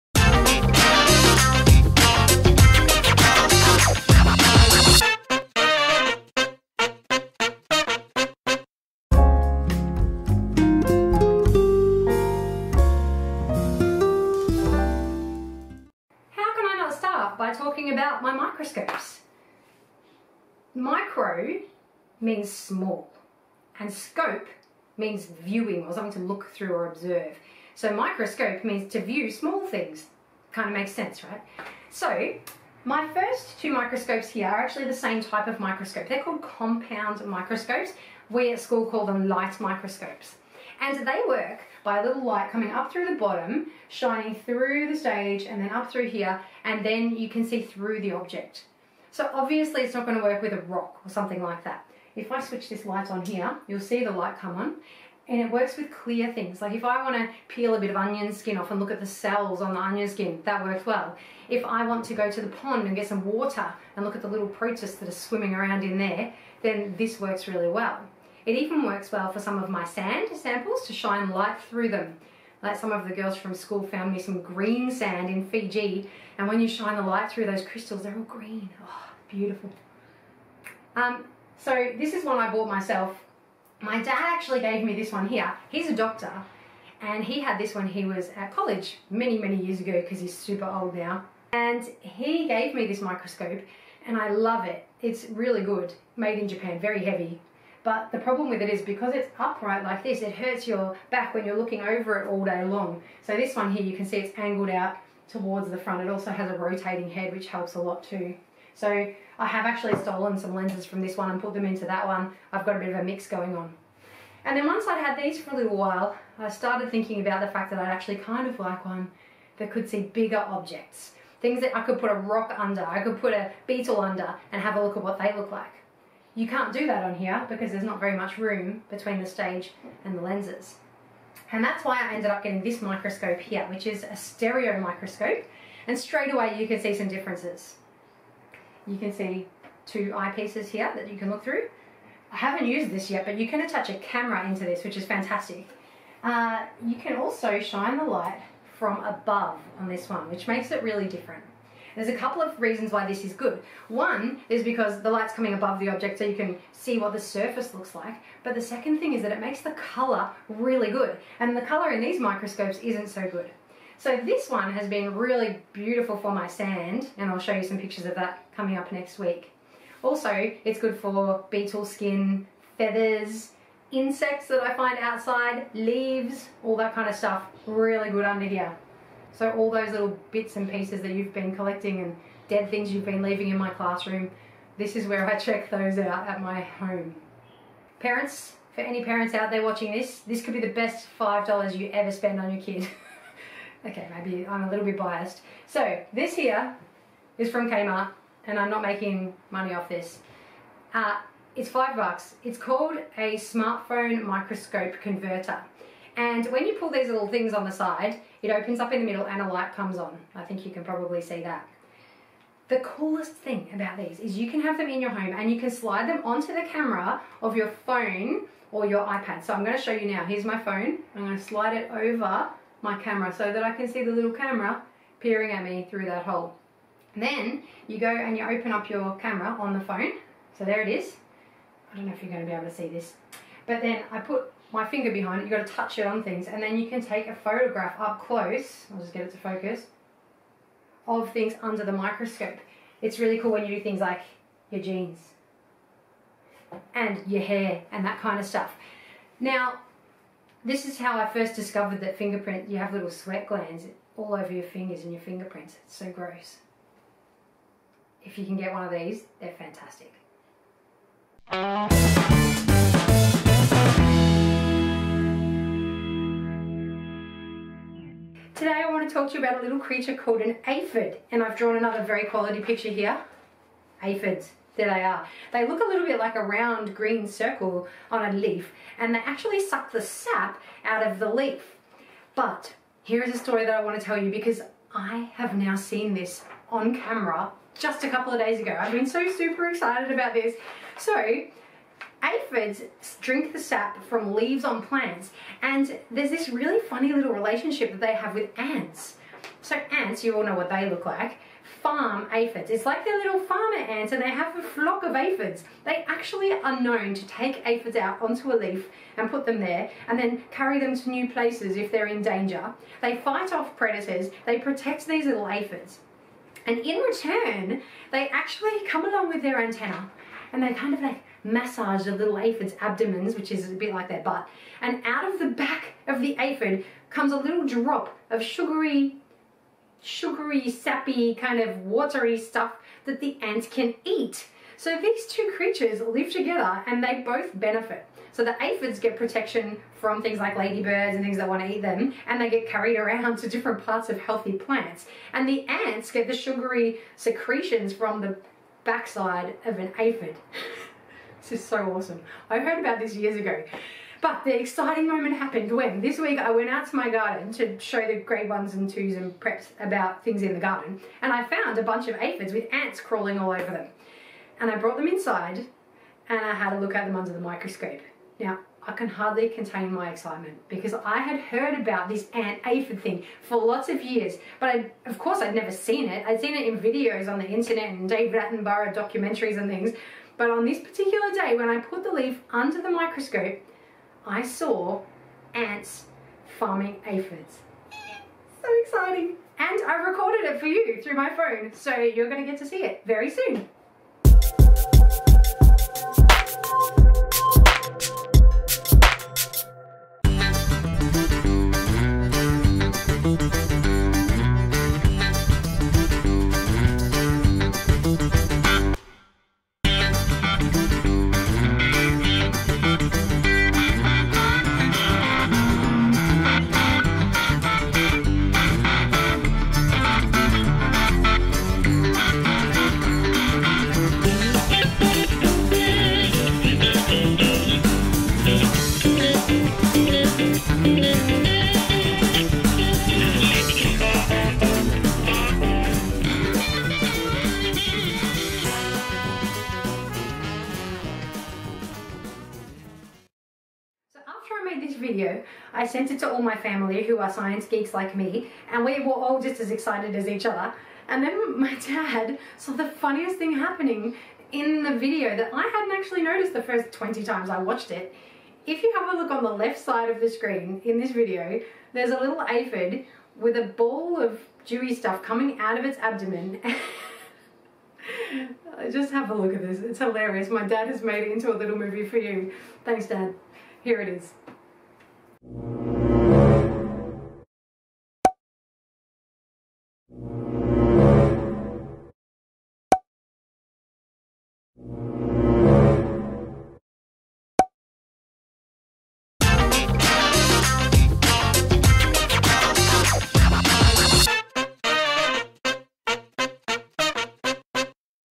3. Interesting objects and aphids. - Foundation - Grade 6 (Science @ Mrs. Mac's house)
Learn a little about microscopes and then see the most exciting footage of Ants farming Aphids.